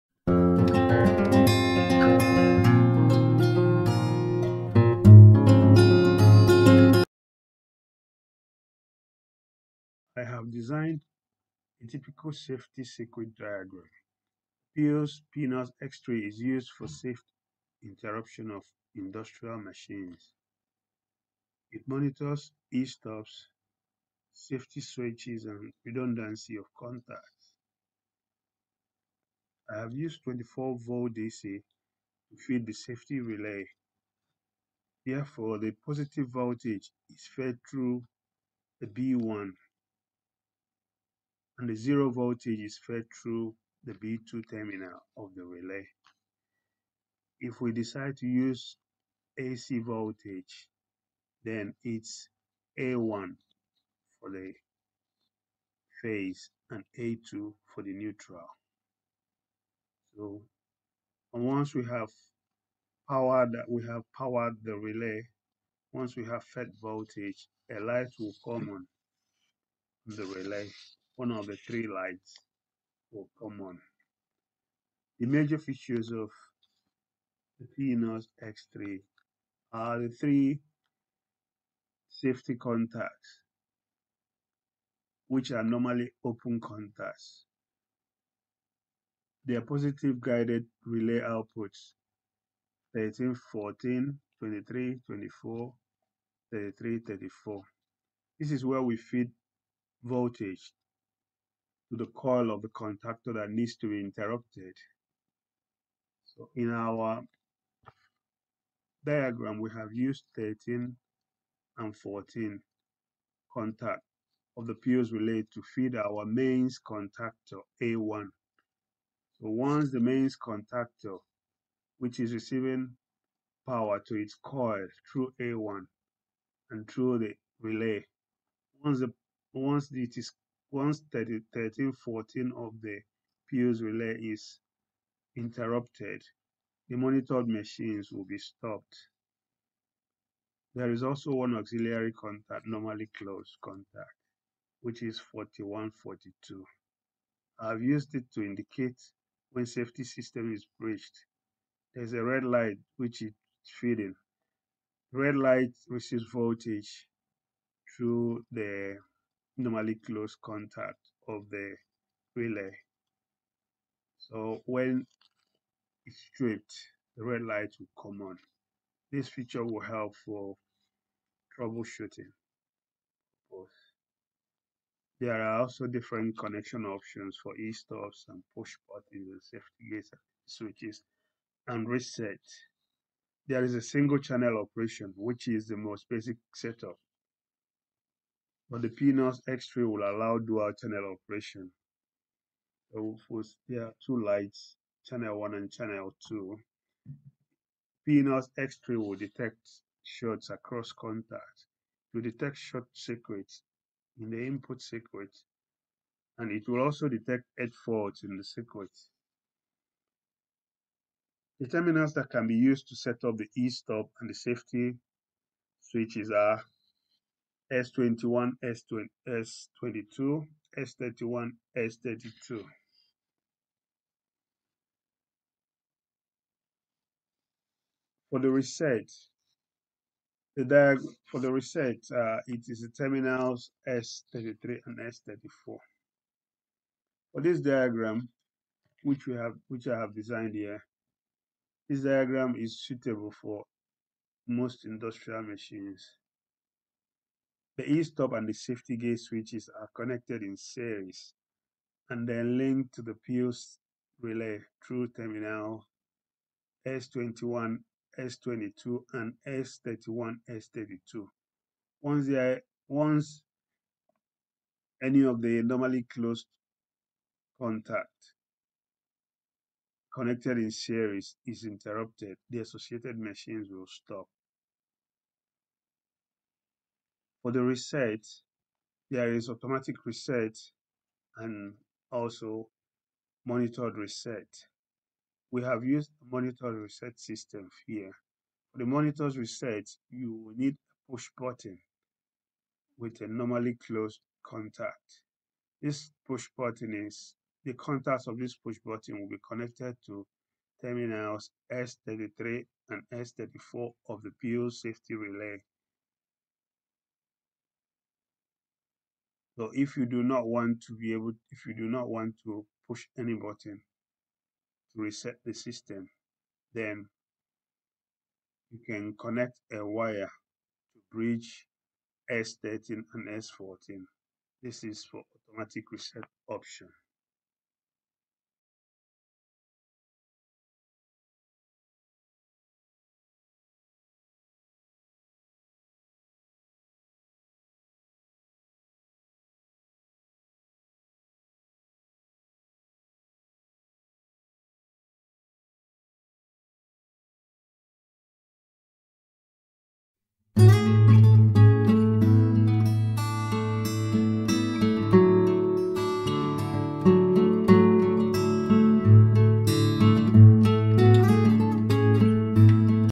How to wire up Pilz Safety Relay Pnoz X3, Safety switches, Estops, Light curtains -Part 1 of 3
Diagram of a typical safety circuit explained with Pilz PNOZ X3